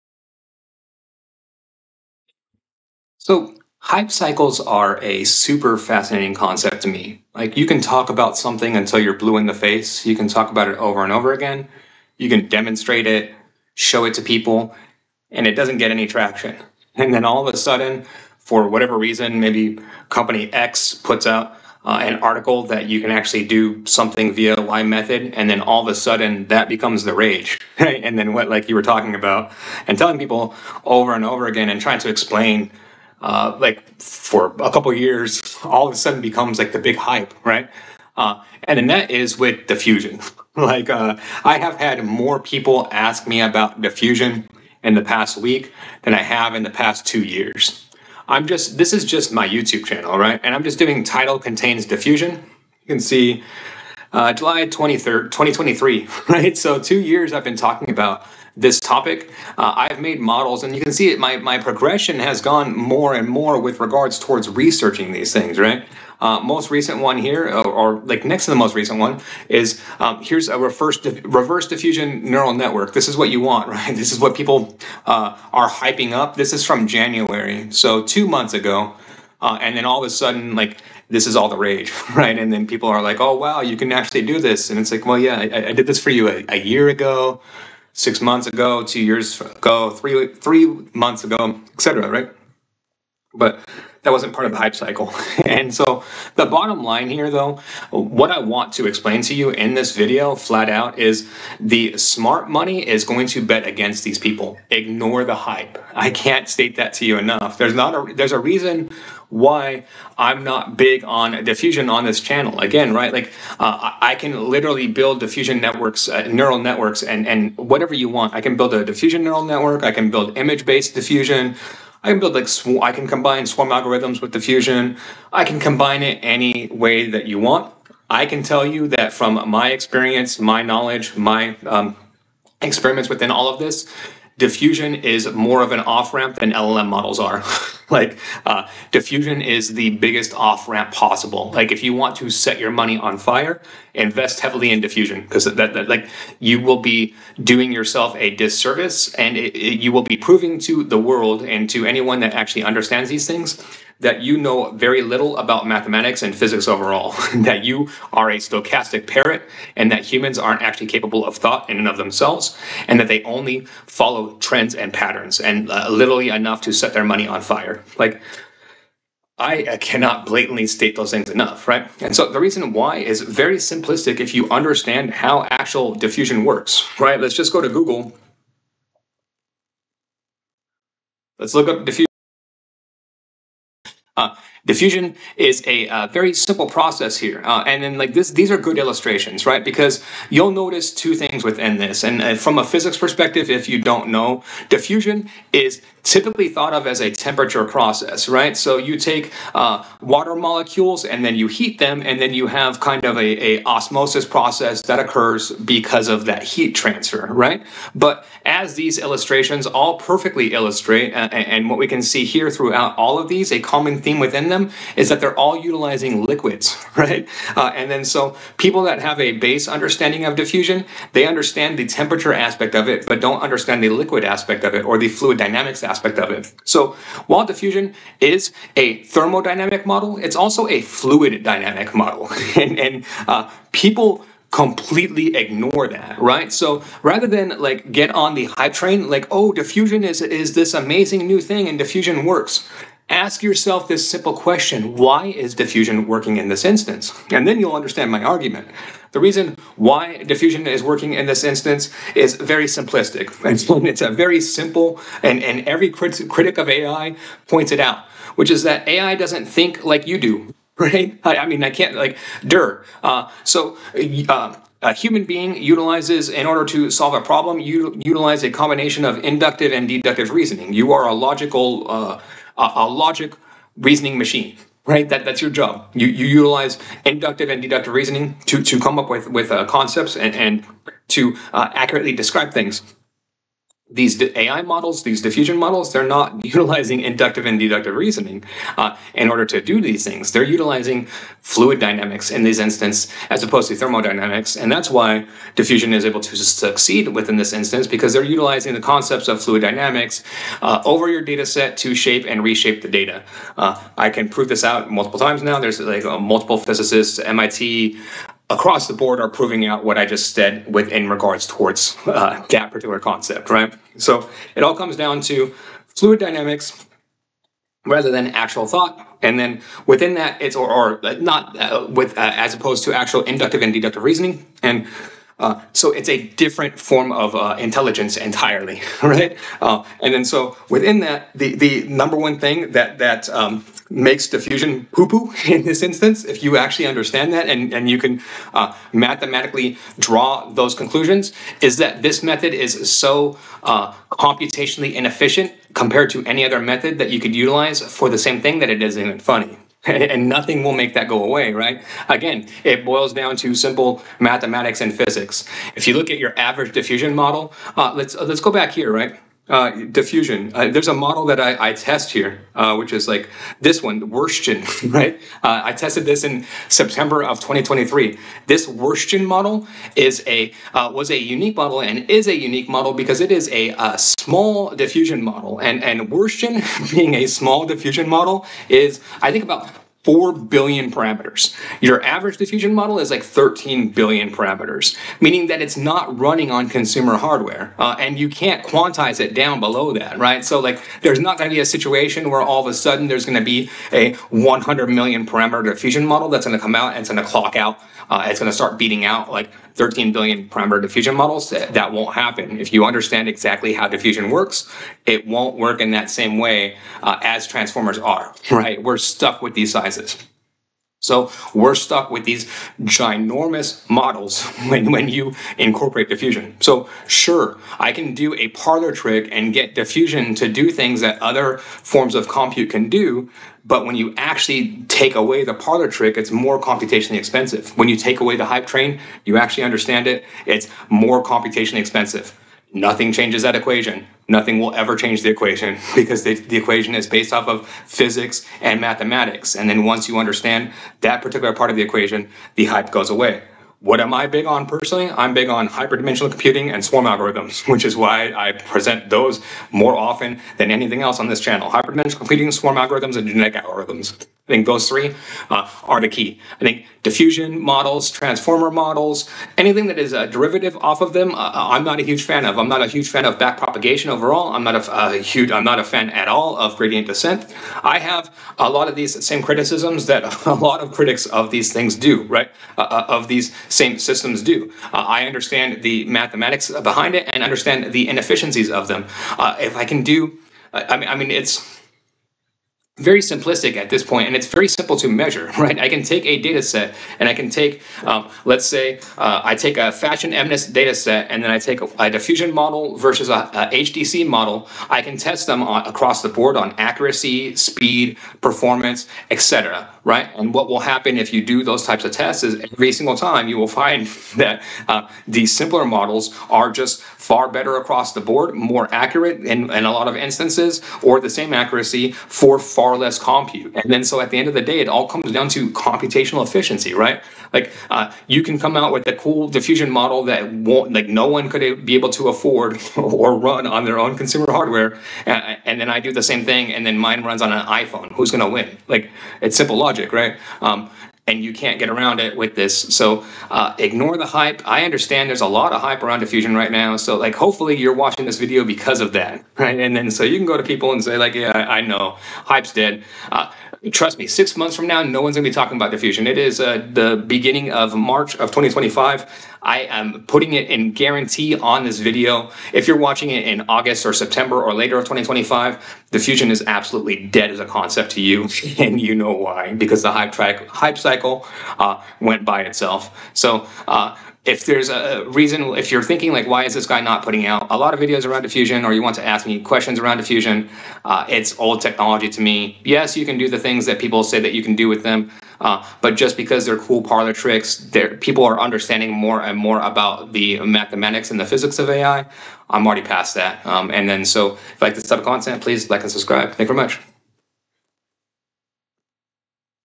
You Should Ignore All Of The Current Hype Around Diffusion
This video discusses hype cycles, using diffusion as an example.

Here's a summary:

The speaker notes that diffusion models have recently become very popular, despite him discussing the topic for two years [00:46].
He advises viewers to "ignore the hype" around diffusion models [01:40].
He argues that diffusion is more of an "off-ramp" than LLM models [02:13].
Diffusion models use fluid dynamics, not inductive and deductive reasoning [04:45].
Diffusion models are computationally inefficient [05:32].
The speaker favors hyperdimensional computing, swarm algorithms, and genetic algorithms [07:22].
He predicts that the hype around diffusion will die down by August or September 2025 [09:04].